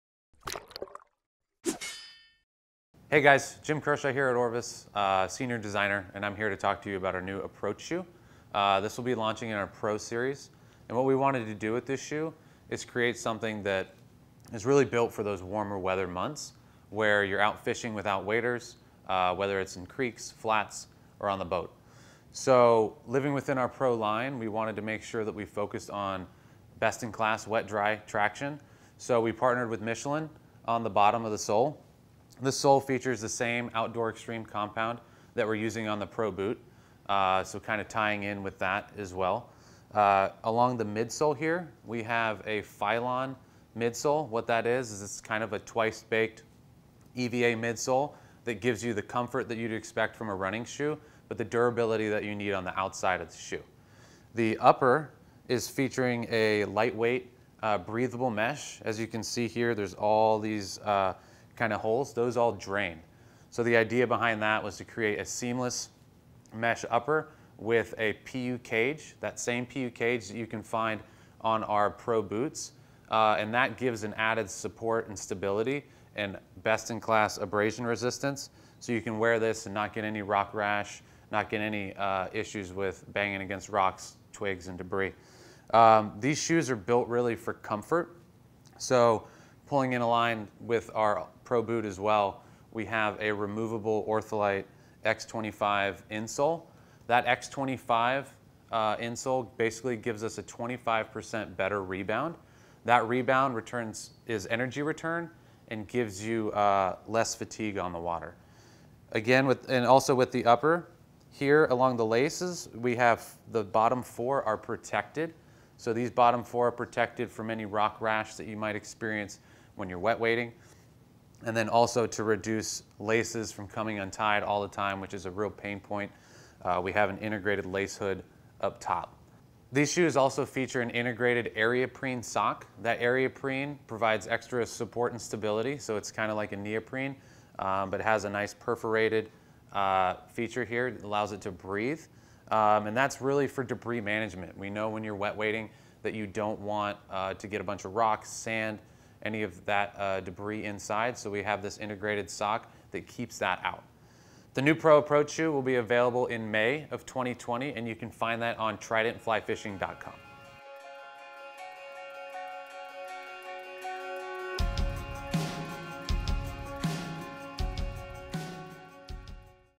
Orvis Approach Shoe | Jim Kershaw Insider Review
Comfortability and performance for your fishy approach...

Orvis has designed a shoe built for the warmer months and for wet wading conditions. The Approach Shoe features a Michelin sole, lightweight breathable mesh upper, and a smooth gravel guard and shoelace garage for extreme comfort and usability. Whether you spend your time on the front of a flats boat or poking around backcountry streams in Montana, the Approach Shoe will make your days on the water more comfortable and more successful.

Got Questions?